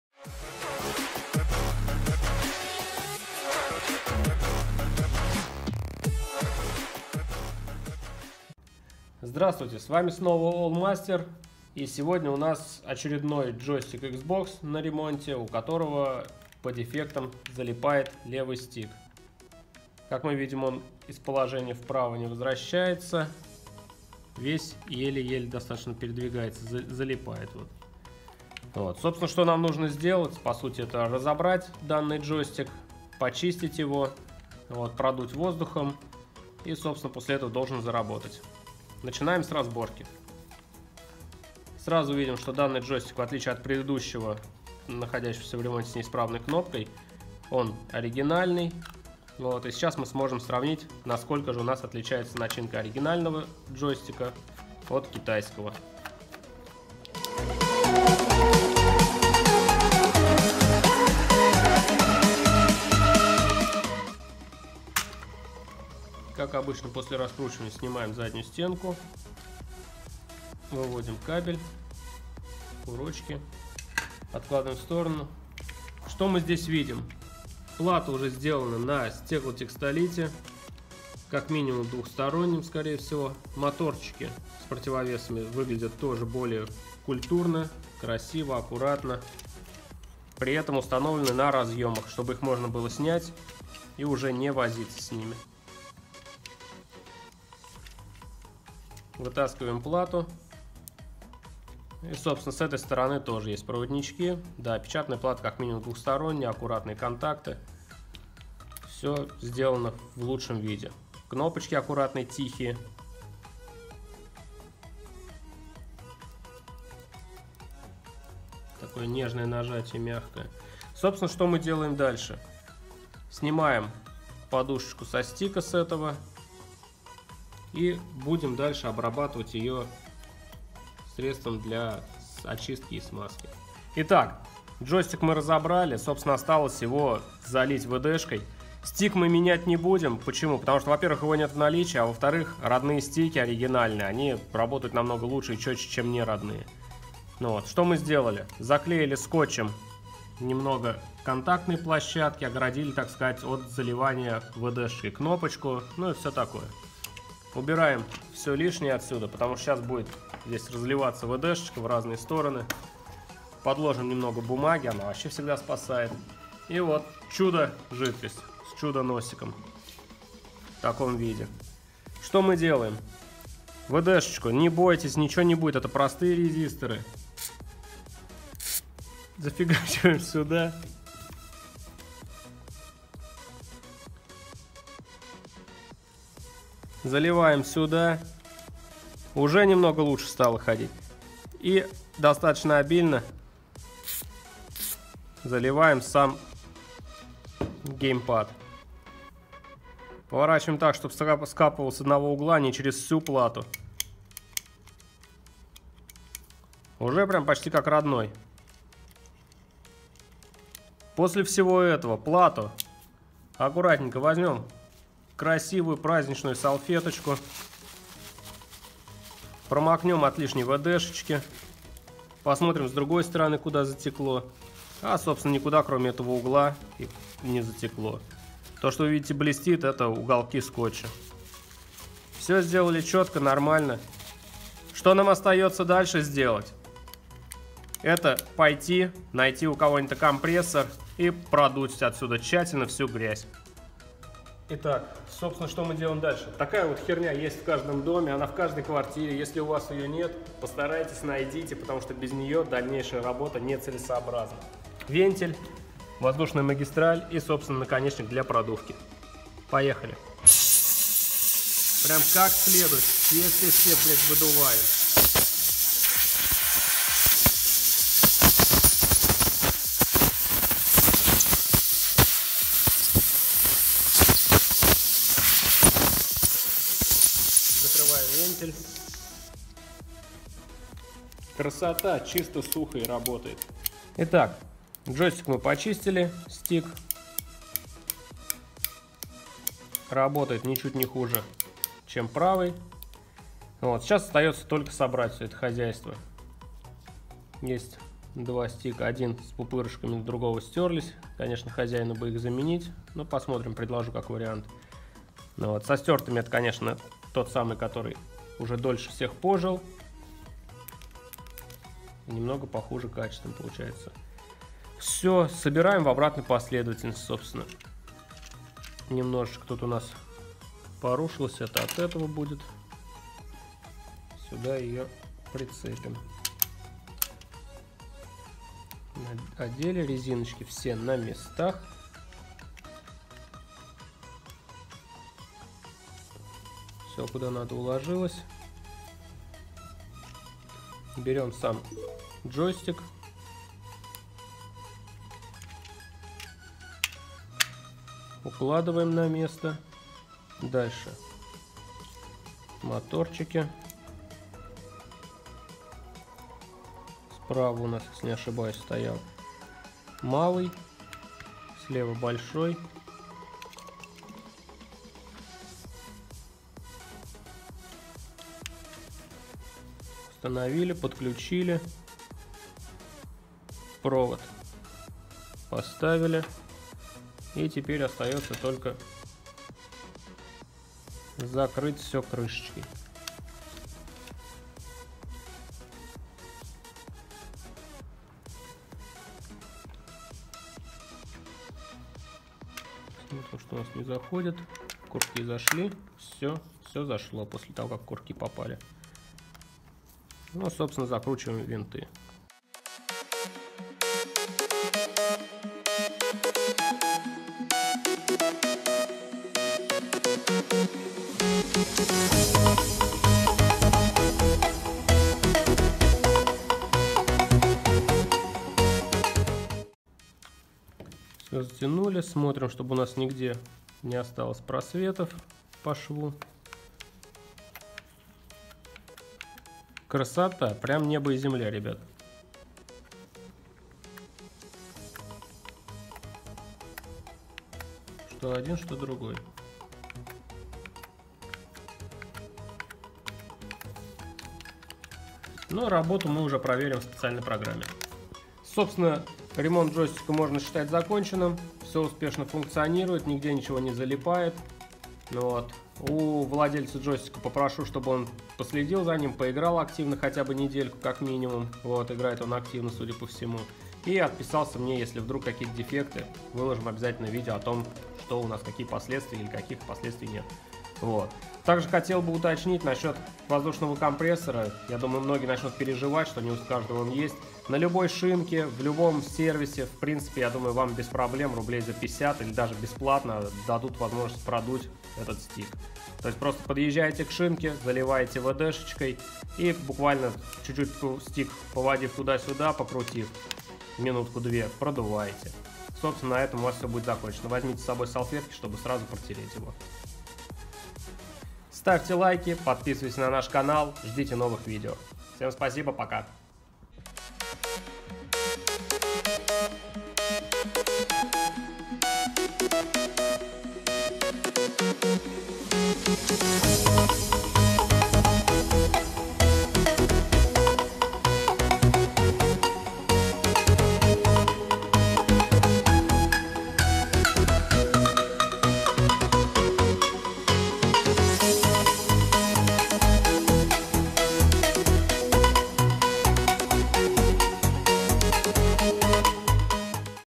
Что делать, если залип стик? Ремонт геймпада XBOX своими руками. Ремонт джойстика.
В данном видео мы подробно расскажем, как быстро, в домашних условиях и своими руками починить залипший стик на геймпаде xbox.
Если есть вопросы ждем в нашем телеграмм-канале: t.me\allmaster152